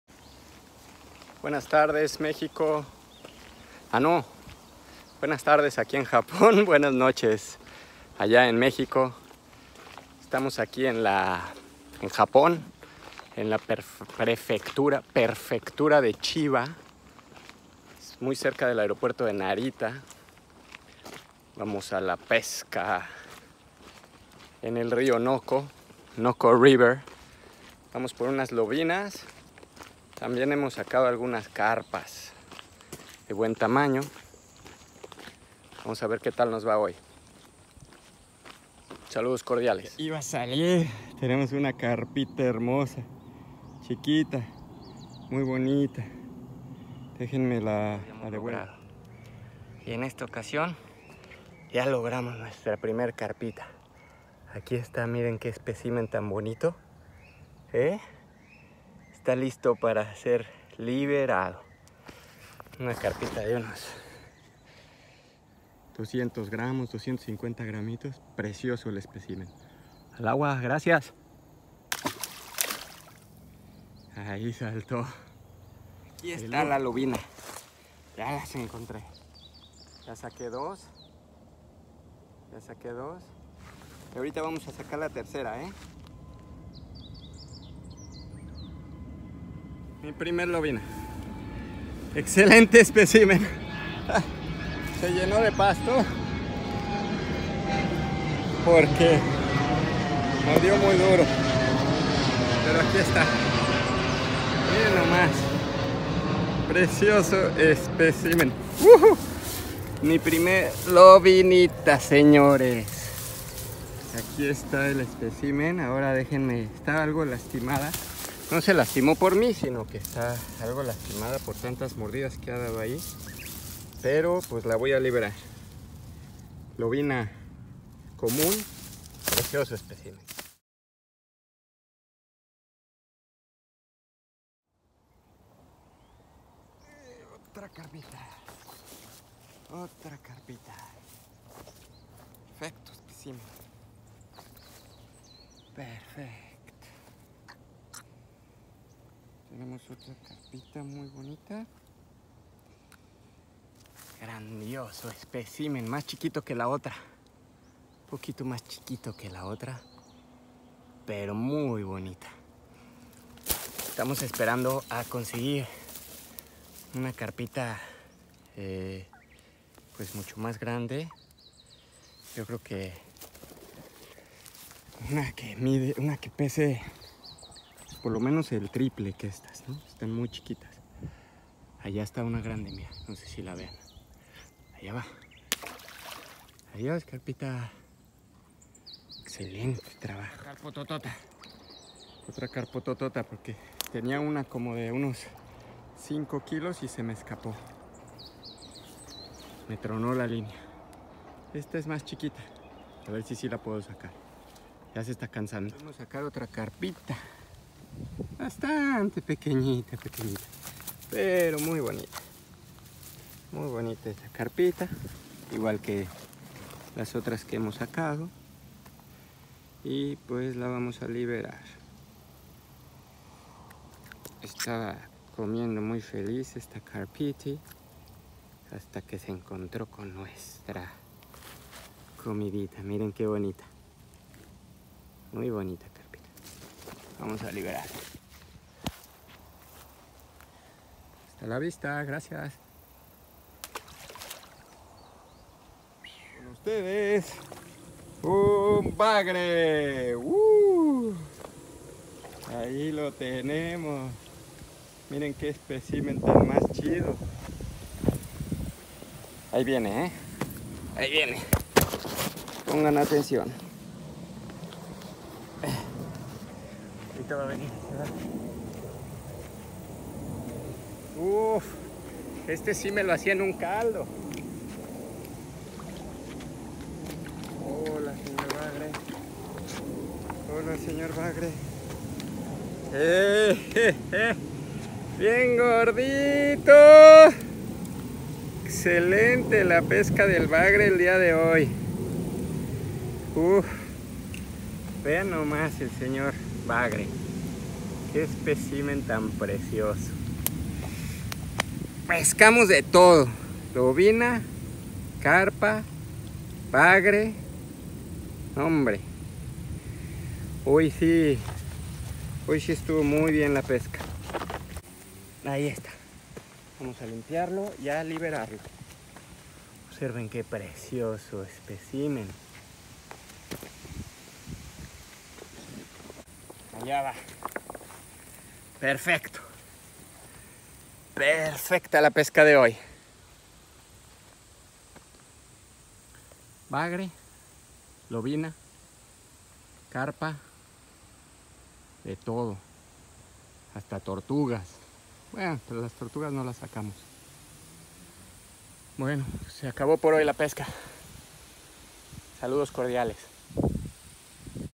Pesca en Japon, mi primer lobina, bass fishing
Yo le llamaba carpa pero es Barbo! Y Perdon por la Mala grabación x ser con un celular.